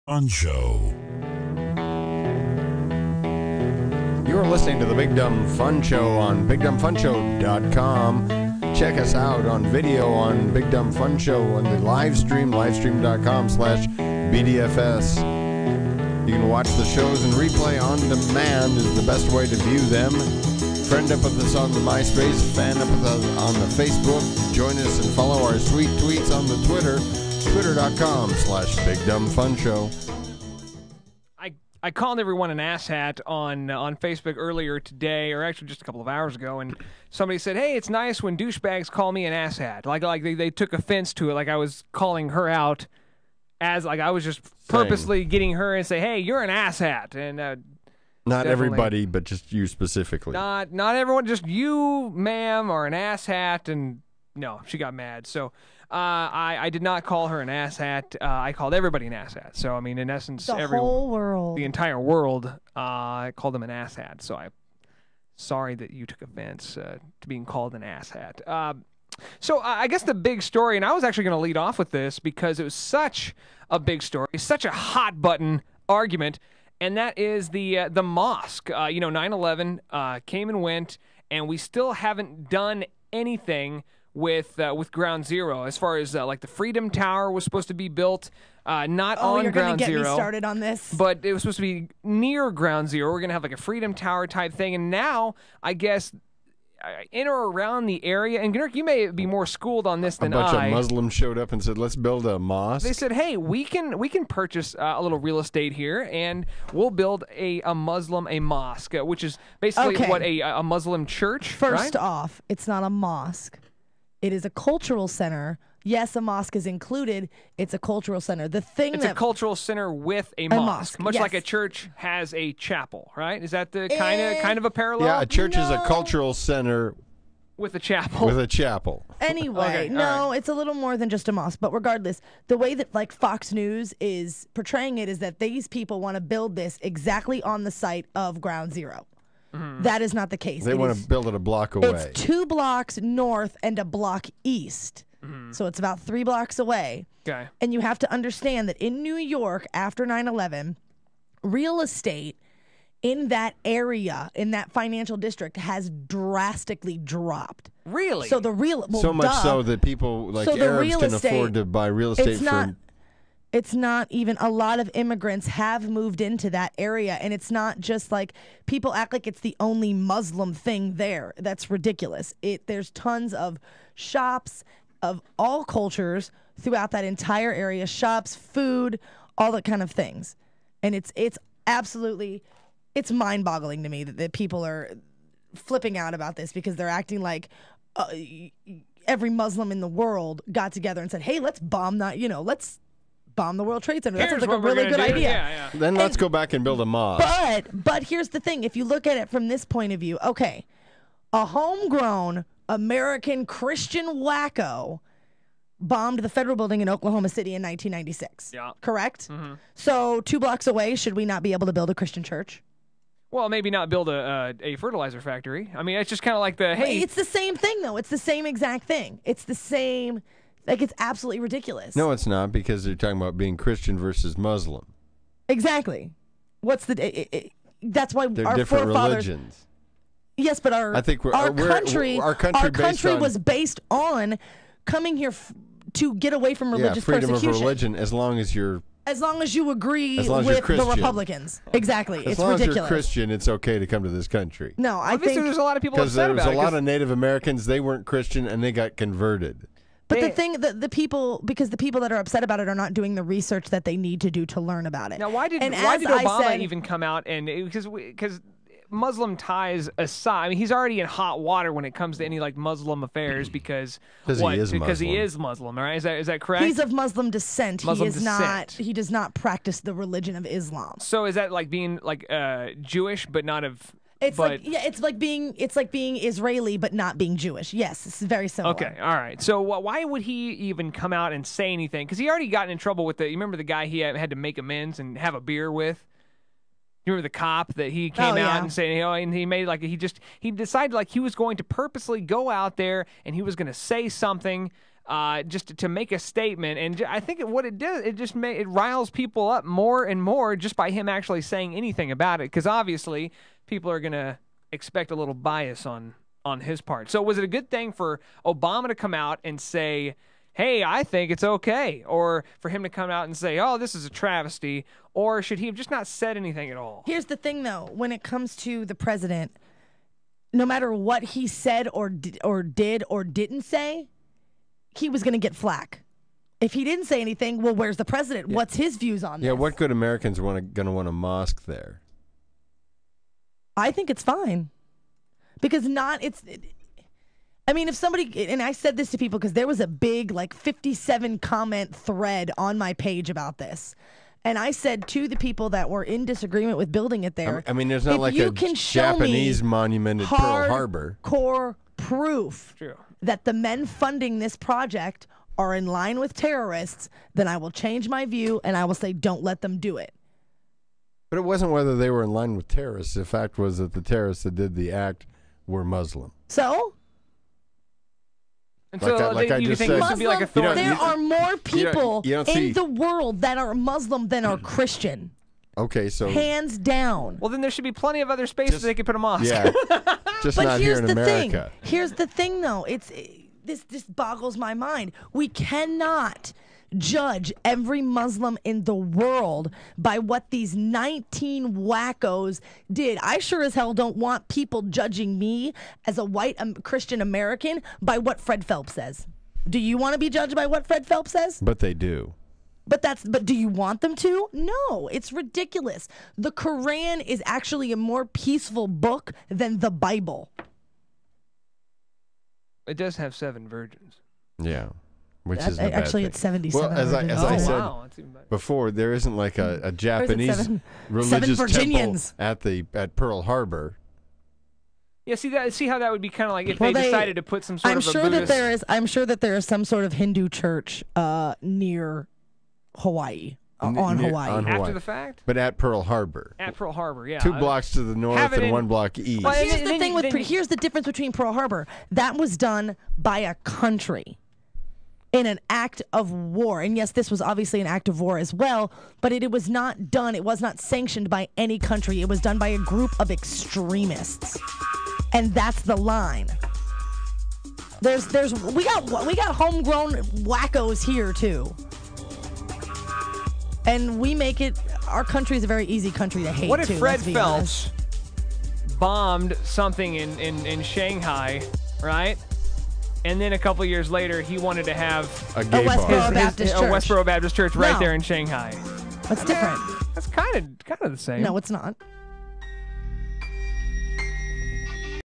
Mosque on Ground Zero?
Should they allow an Islamic Mosque near Ground Zero? Applesnatch Jaxon is passionate about Islamic rights. Venkman could care less either way.